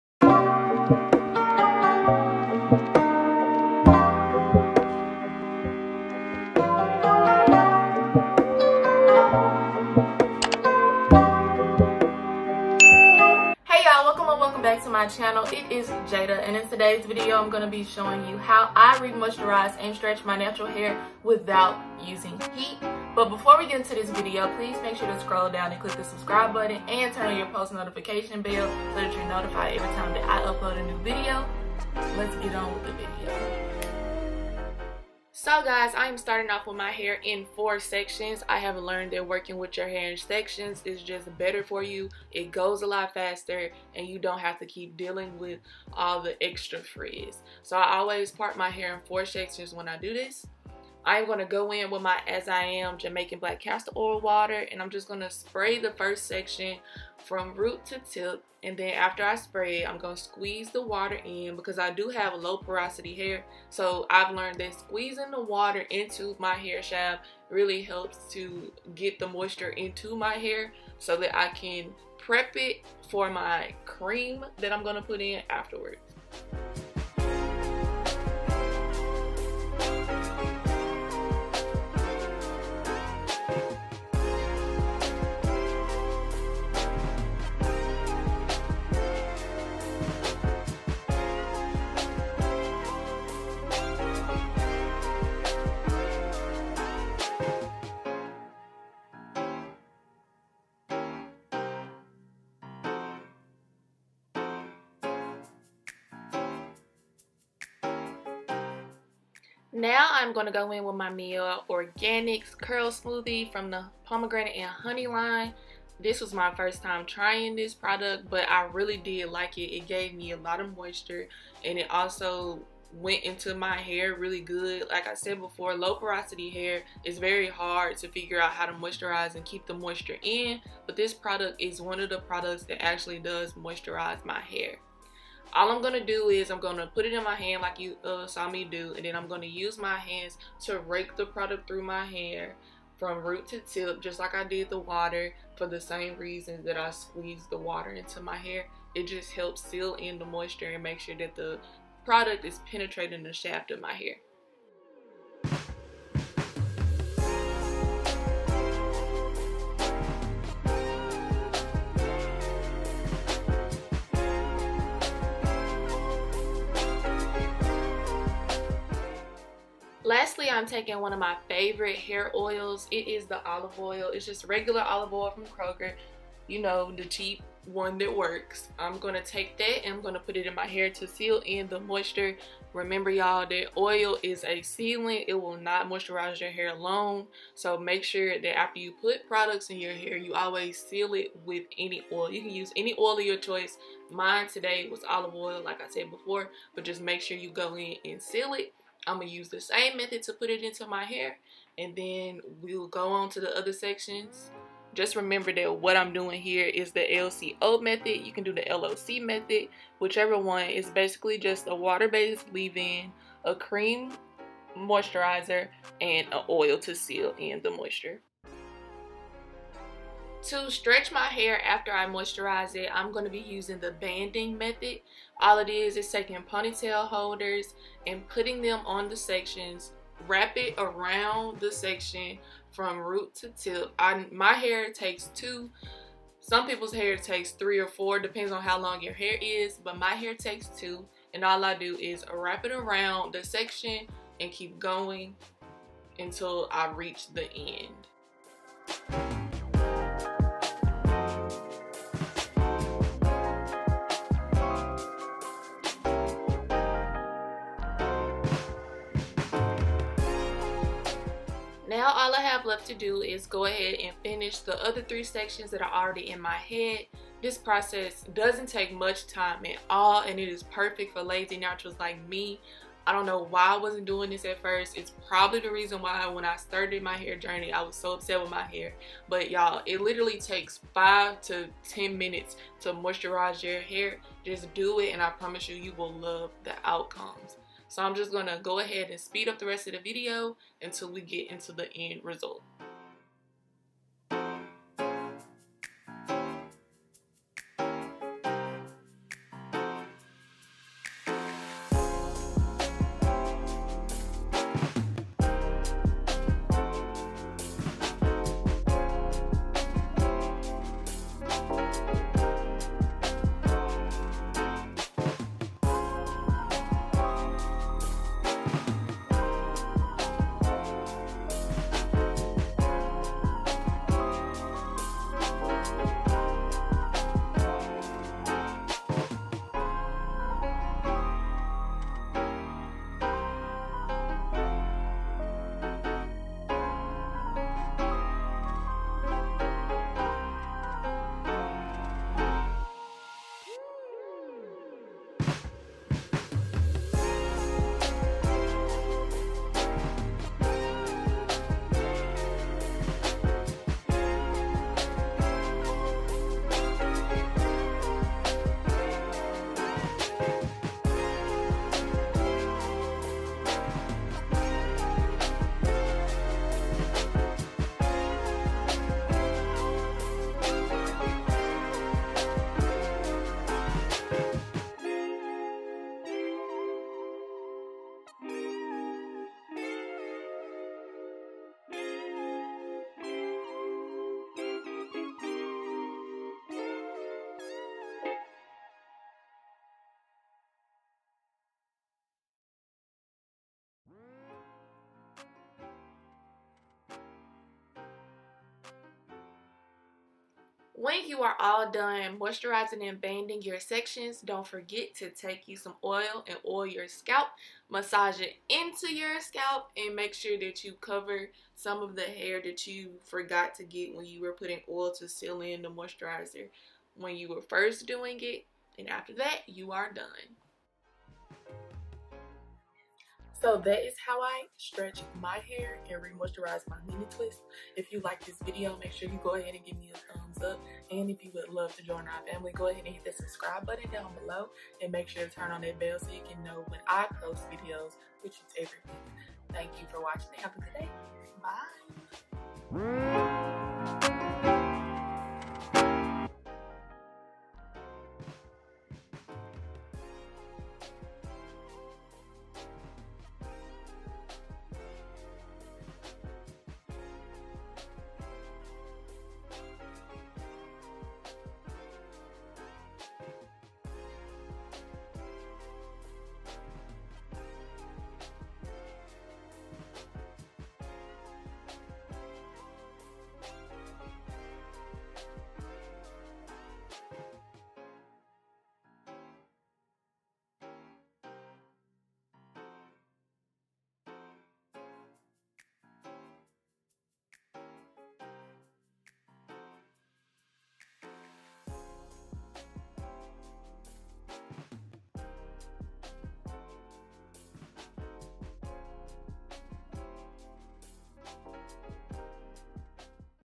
Maintaining Mini Twists | + How to Stretch twists| JadaElle
Hey yall! I'm back with a new video. Today's video is a about how i maintain mini twists until my next was day.